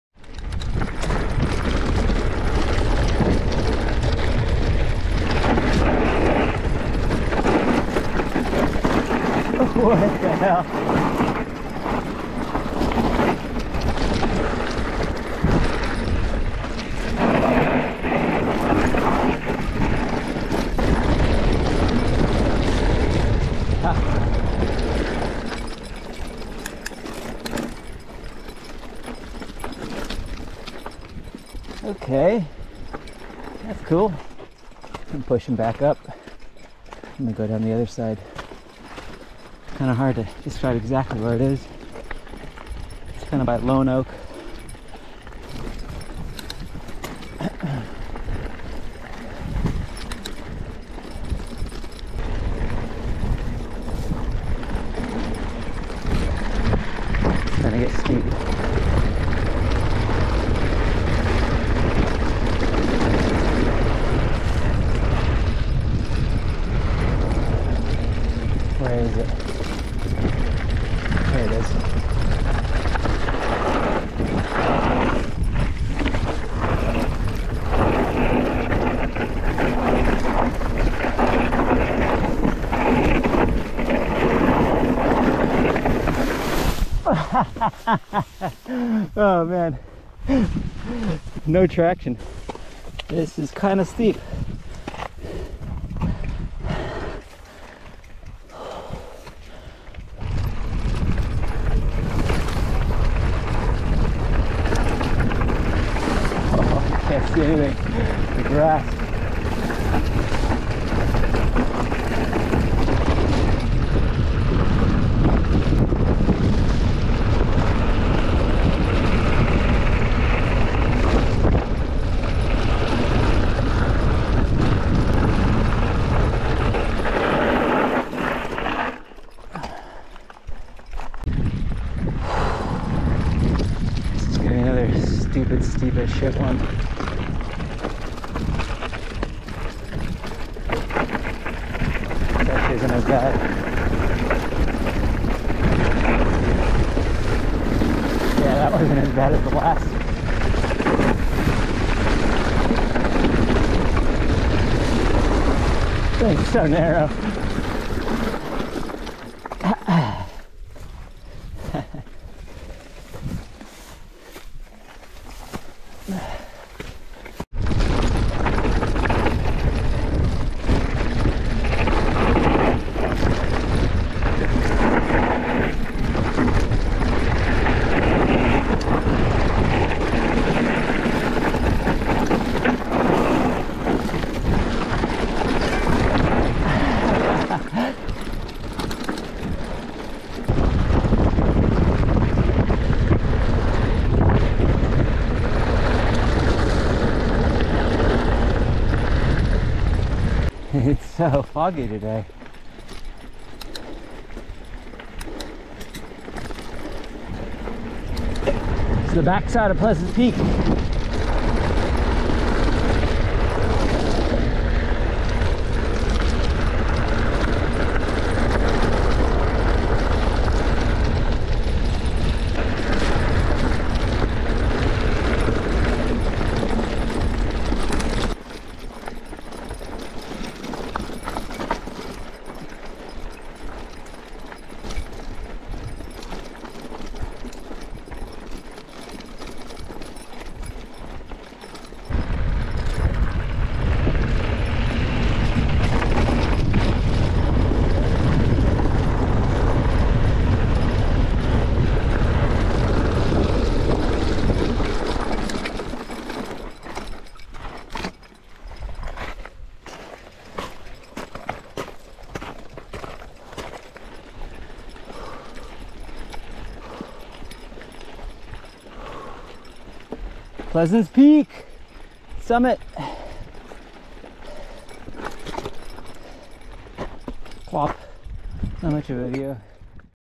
Exploring Santa Ana Singletracks Off Main Divide
There are a multitude of unnamed short singletrack dirt biker trails that drop off and back onto Main Divide all across the Santa Anas. Here are just a few of these little shorties near Ladd Springs, Lone Oak and Pleasant's Peak. Next time you're out on the Main Divide and see a little singletrack off to the left or right, roll the dice and see where it takes you. Probably back on to the road!